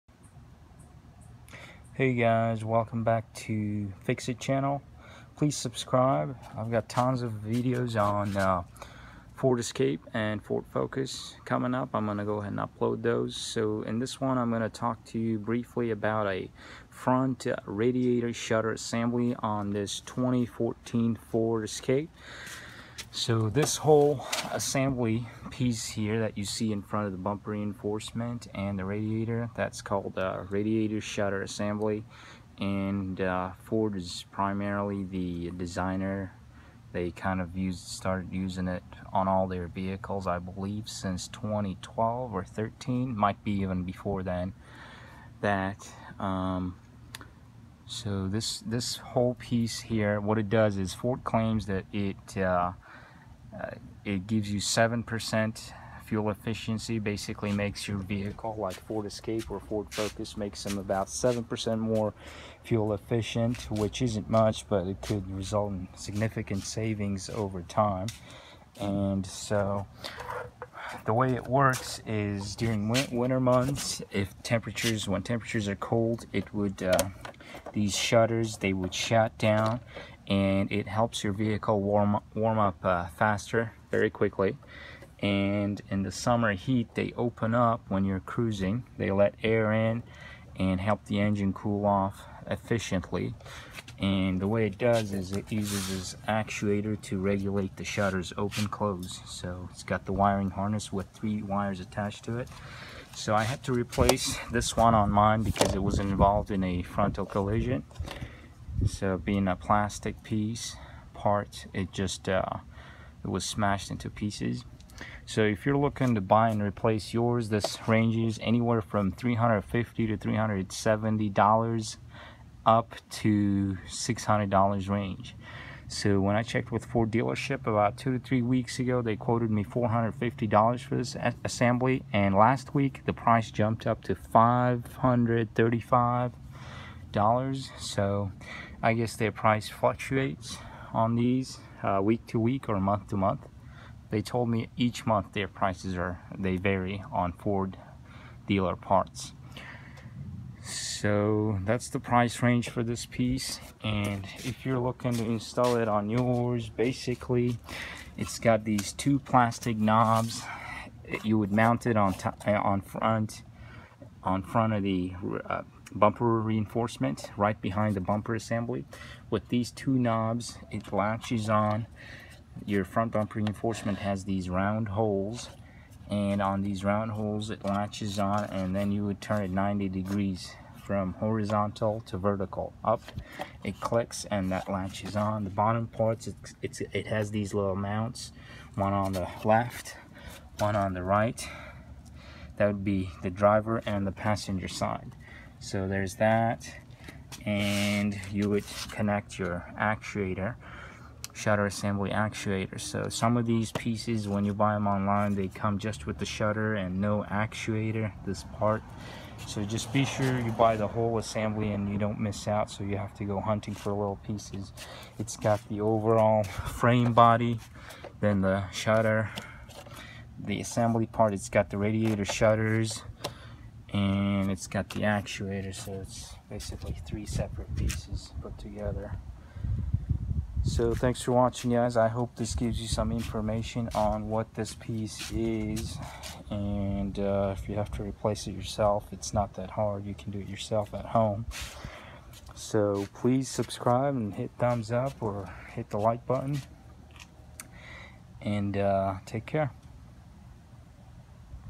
2014 Ford Escape Radiator Shutter Assembly Installation Explained
In this video I just show the Radiator Shutter Assembly after I installed it on my 2014 Ford Escape 1.6 Turbo.  I explain the installation process briefly but do not show it step by step.